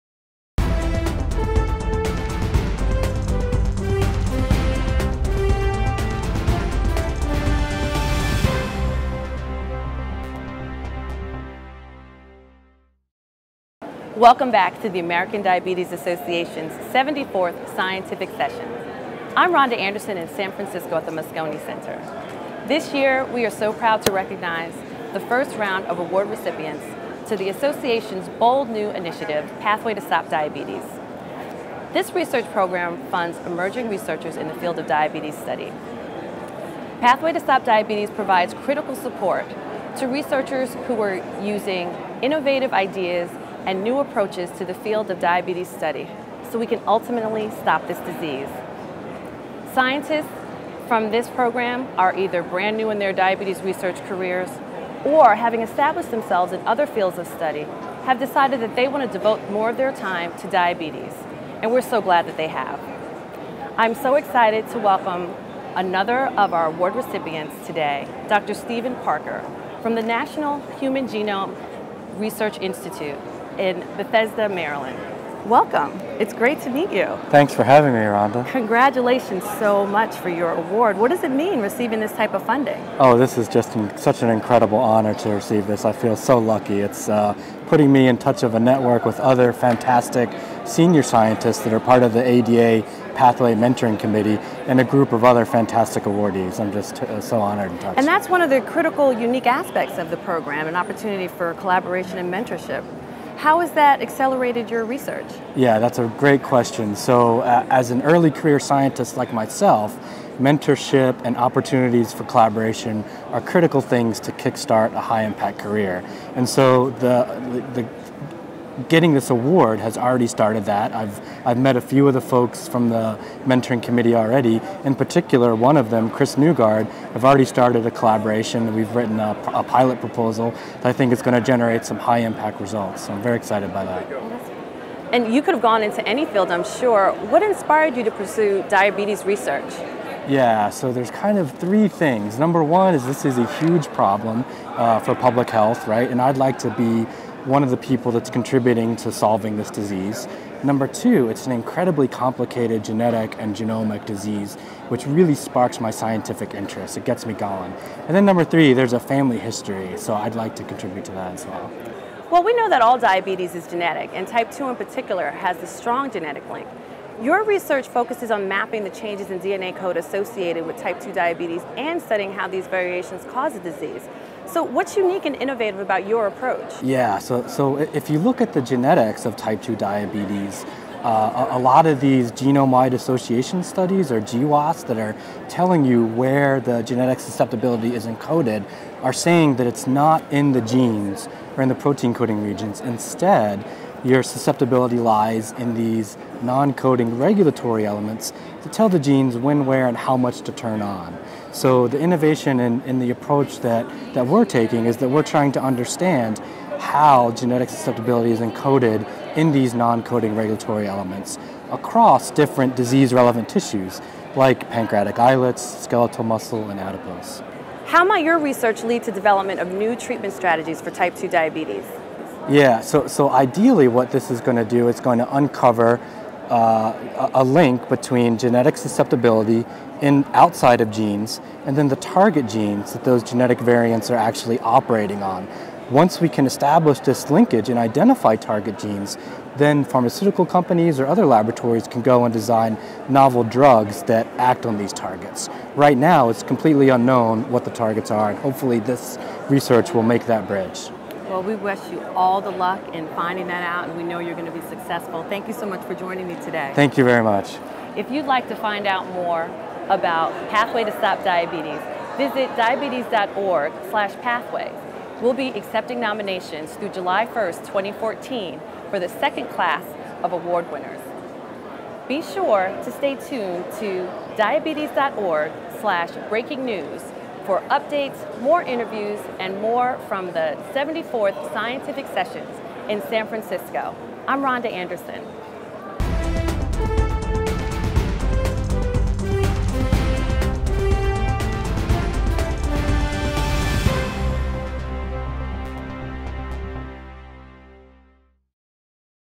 74th Scientific Sessions: Interview with Researcher Dr. Stephen Parker
From the 74th Scientific Sessions in San Francisco, Rhonda Anderson interviews Stephen Parker, PhD, one of the first recipients of the Pathway to Stop Diabetes research grants. His work focuses on mapping the changes in DNA code associated with type 2 diabetes and studying how these variations cause the disease.